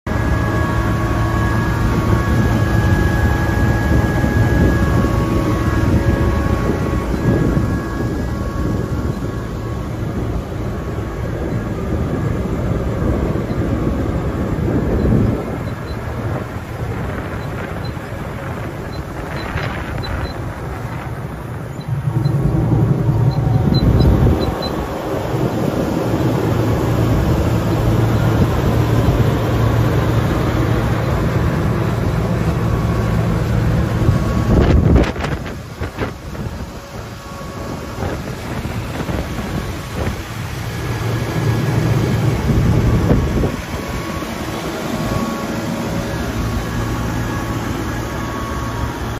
2011 TERRA SELECT W7 For Sale
Terra Select W7 Wind Sifter

Designed for cleaning light impurities, such as plastic out of screened compost and biomass materials.

Variable Speed, feed conveyor
Wind Sifter
Diesel Engine
Stone Trap
Roller Magnet
Technical Specifications

Motor 108hp
Type Diesel
Fuel Tank 180L
Hydraulic Tank 80L
Hopper Width 3650mm
Hopper Length 3660mm
Operating Length 13,620mm
Operating Width 2,550mm
Operating Height 4,000mm + extra side boards on the hopper
Weight 12,000kg